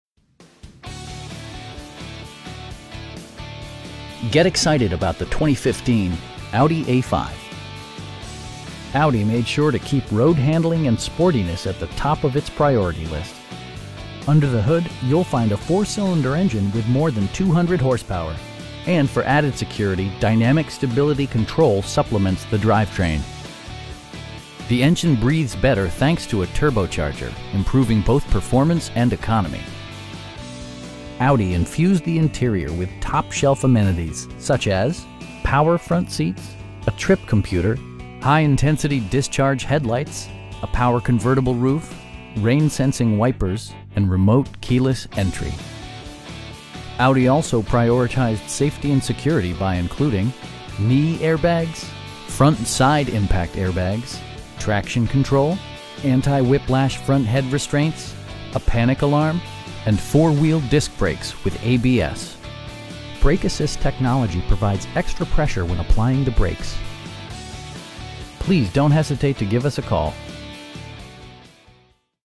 2015 Audi A5 2.0T Premium (Tiptronic) in Dedham, MA 02026
Get excited about the 2015 Audi A5. Audi made sure to keep road-handling and sportiness at the top of it's priority list. Under the hood you'll find a 4 cylinder engine with more than 200 horsepower, and for added security, dynamic Stability Control supplements the drivetrain. The engine breathes better thanks to a turbocharger, improving both performance and economy. Audi infused the interior with top shelf amenities, such as: power front seats, a trip computer, front bucket seats, high intensity discharge headlights, a power convertible roof, rain sensing wipers, and remote keyless entry. Audi also prioritized safety and security by including: knee airbags, front side impact airbags, traction control, anti-whiplash front head restraints, a panic alarm, and 4 wheel disc brakes with ABS. Brake assist technology provides extra pressure when applying the brakes. Our aim is to provide our customers with the best prices and service at all times. Please don't hesitate to give us a call.